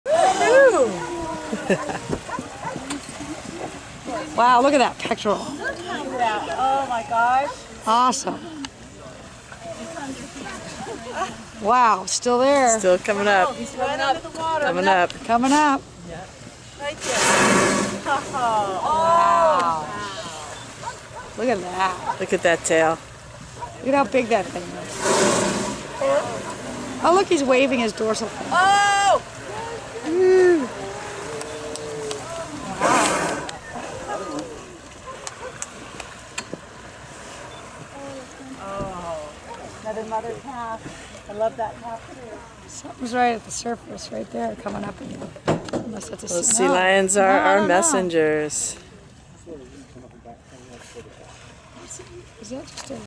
Humpback Whales of Monterey Bay
A truly remarkable day on the Bay whale watching with Sanctuary Cruises. on our next whale watching adventure to Monterey Bay, Baja and beyond!